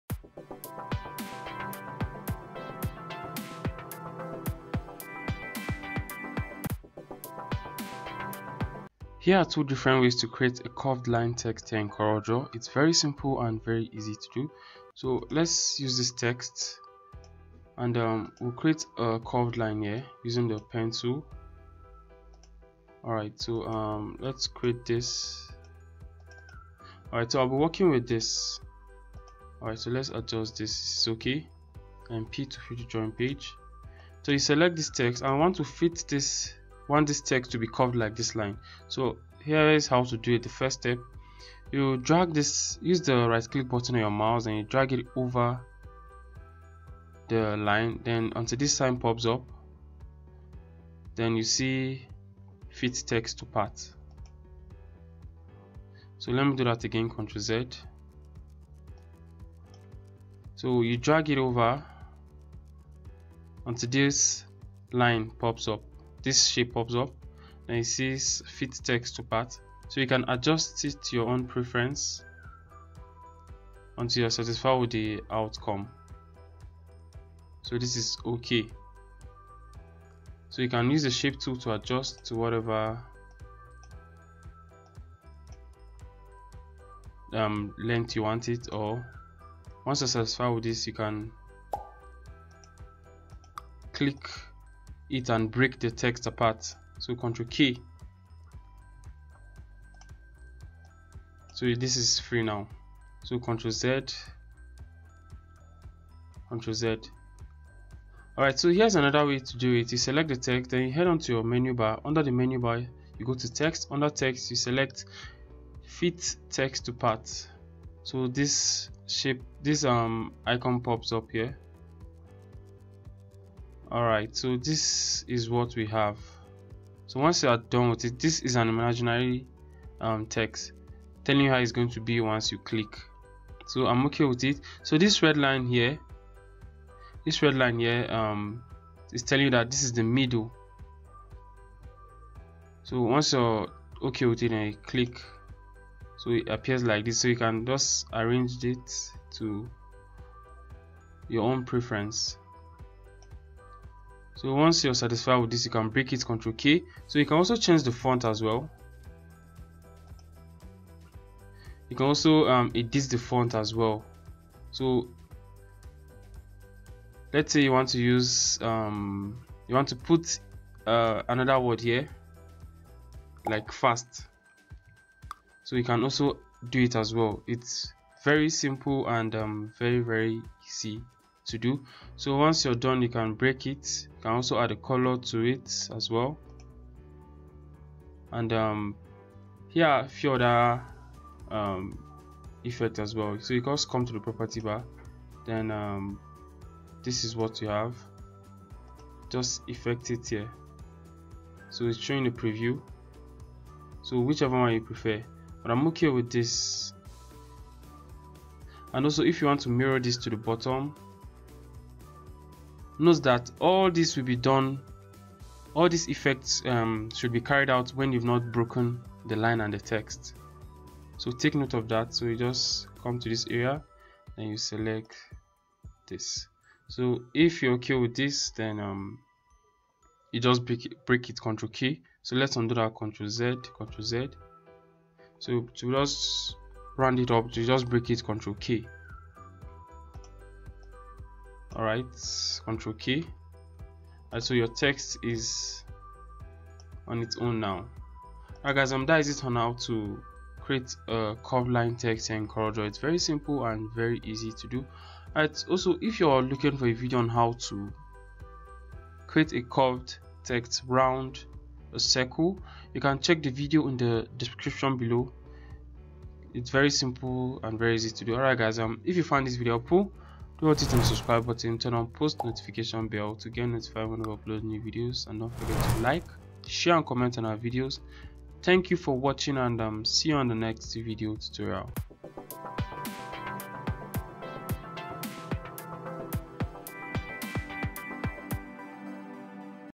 How To Create A Curved Line Text In CorelDraw
Learn easy steps on how to easily create a curved line text using CorelDraw.
It is very simple and easy to do.

HOW TO CREATE A CURVED TEXT IN CORELDRAW